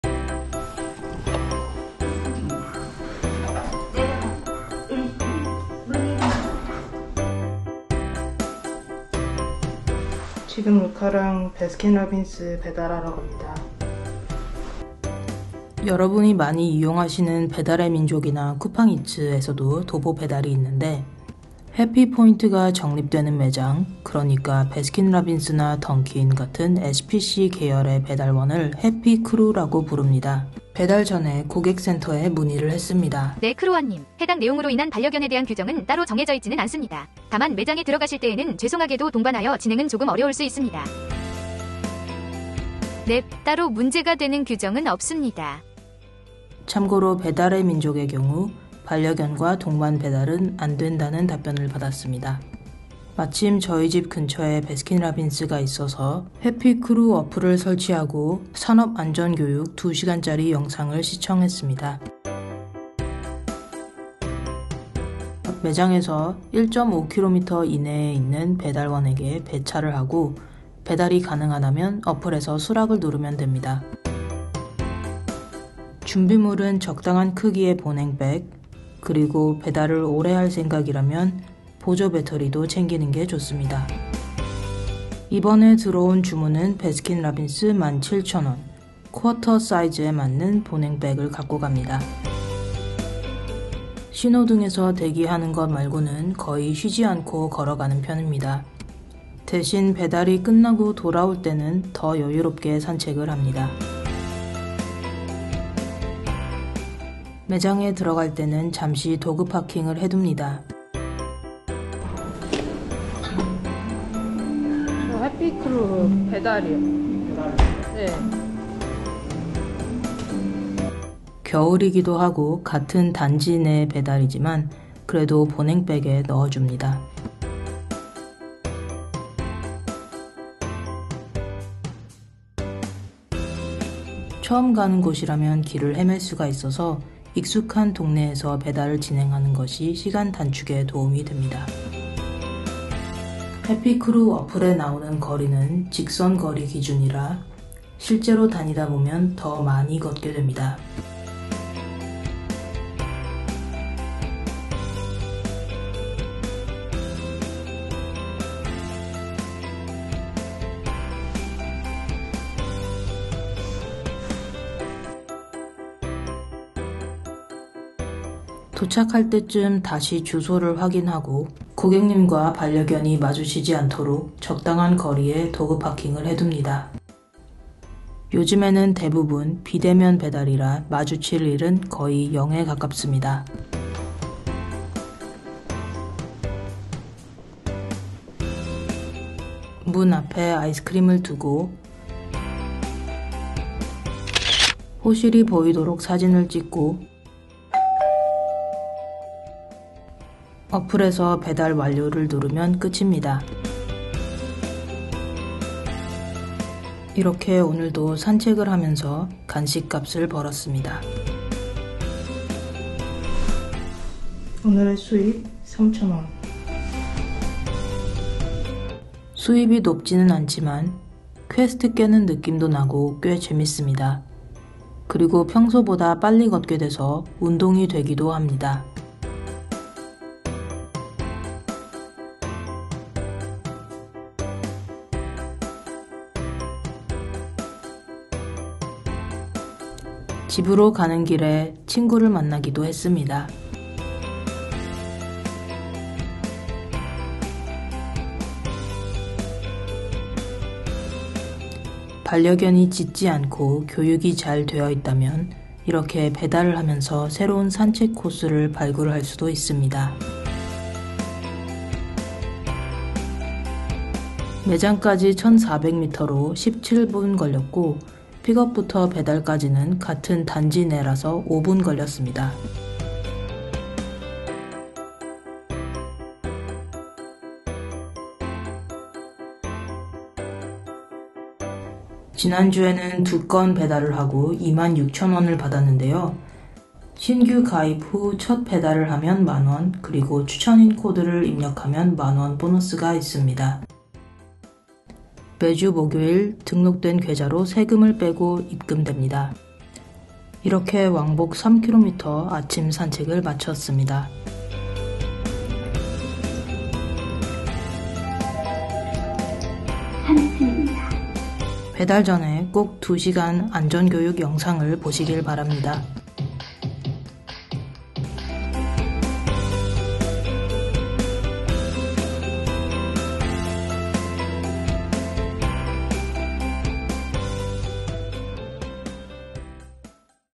산책하며 돈 번다개 | 강아지와 배달알바 | 해피크루 수입
해피크루 고객센터에 문의 후 루카와 같이 배달을 했어요.
루카의 해피크루 추천인코드 : HC10246321

@happypoint_official 해피독크루가 되고 싶다개!